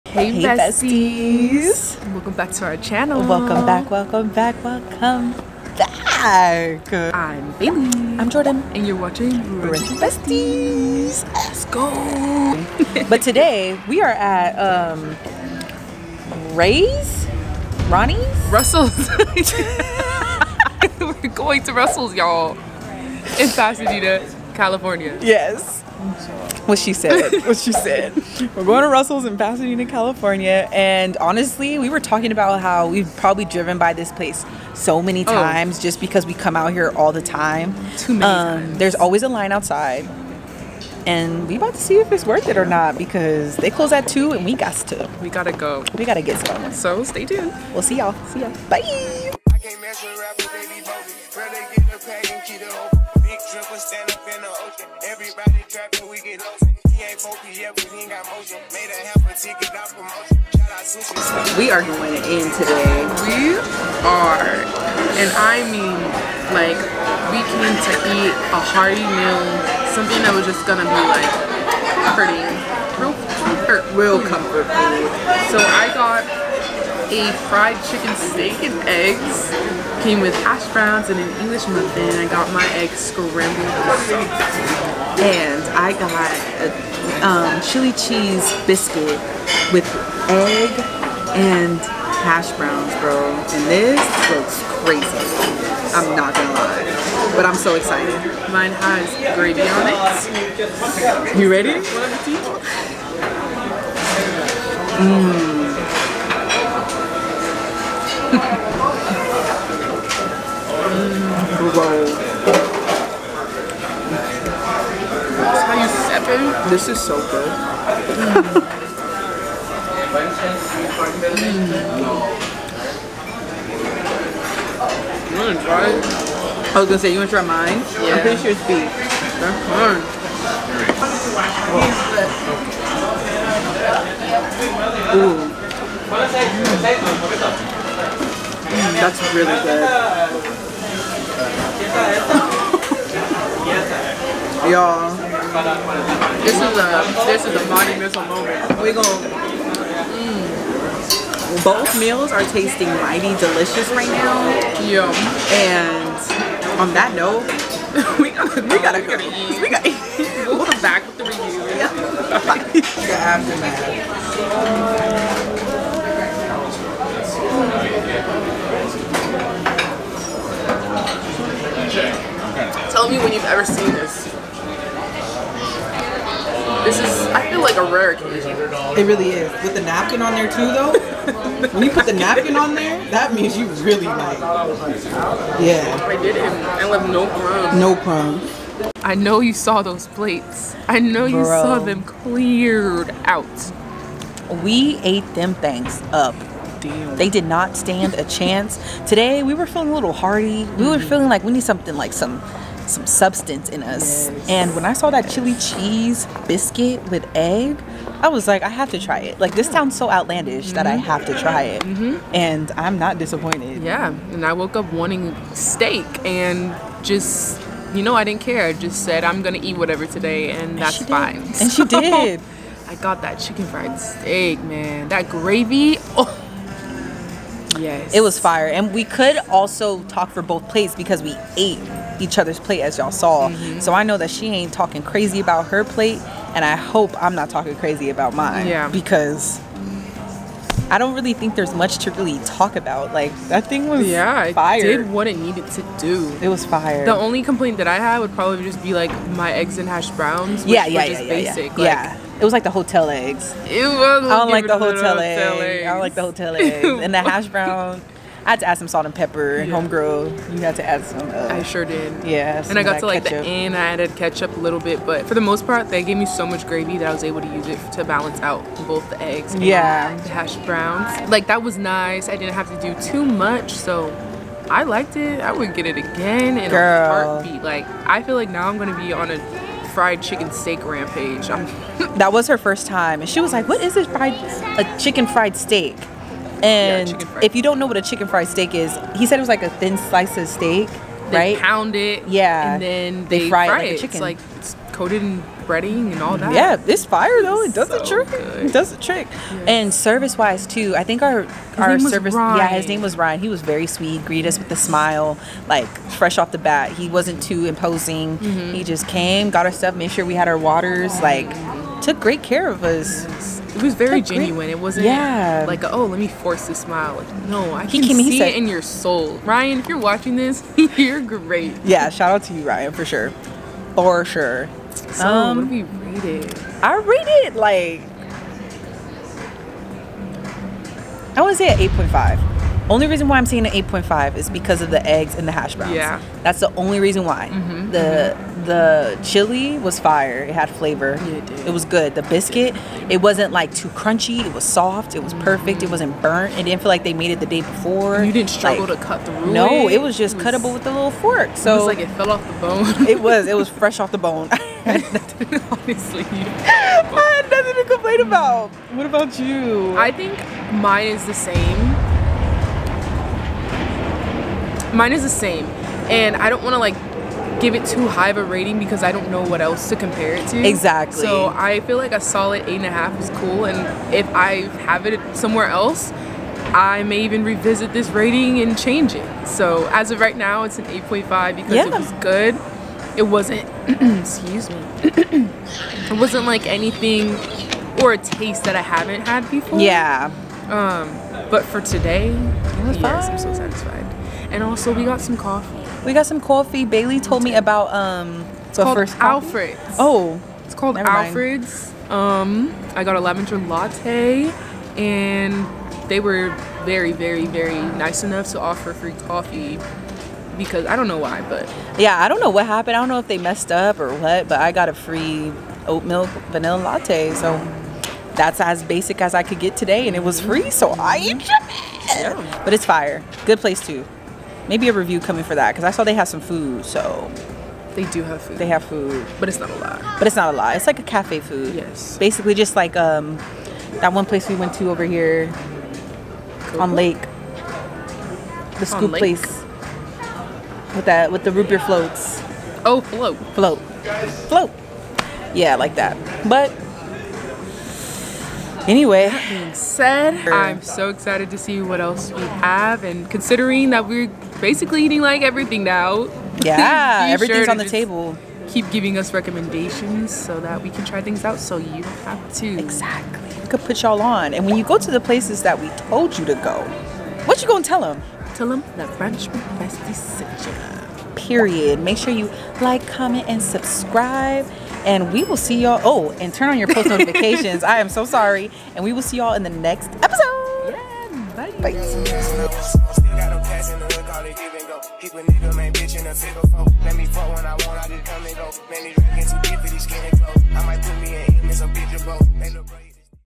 This Food Had Us Acting DIFFERENT At This Brunch Spot | Food Review | Brunch W. Besties 🫶🏽
BWB 🤝🏽 Comfort Food

We were feeling really hungry and needed the most filling brunch we could find so we went to Russell’s in Pasadena, CA

Y’all know we love big portions and a nice savory moment! Did it smack though?

Comment which meal you would order🤭

What we ordered:
Chicken fried steak & eggs
Chili and cheese biscuits & egg breakfast